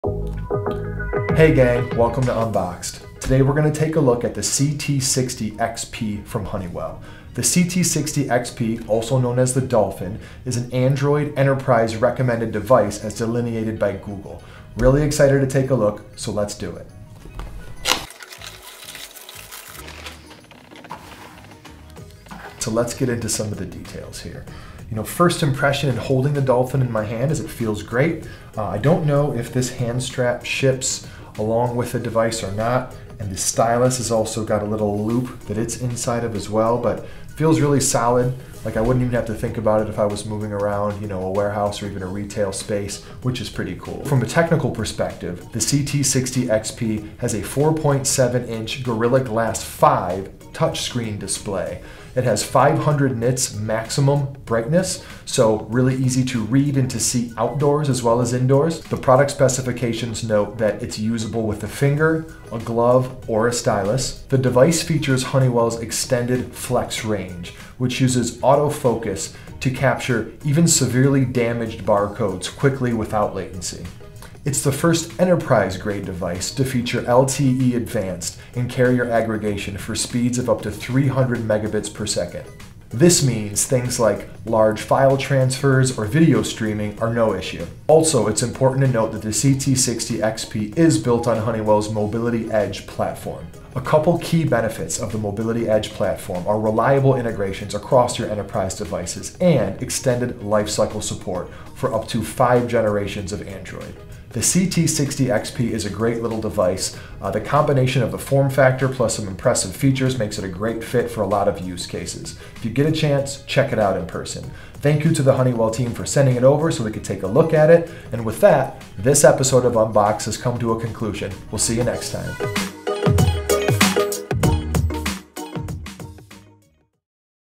Unboxed with the Honeywell CT60 Dolphin Mobile Computer
Honeywell CT60 Mobile Computer is an android enterprise recommended device. It features a 4.7" Gorilla Glass 5 touchscreen with 500 nits of max brightness. It can be used with a finger, gloves, or a stylus. It's the first enterprise-grade mobile computer with LTE-advanced and carrier aggregation which means tasks such as large file transfers and video streaming are no problem.

The CT60 XP is built on Honeywell's Mobility Edge platform to ensure reliable integrations and device lifecycle support for 5 generations of Android OS.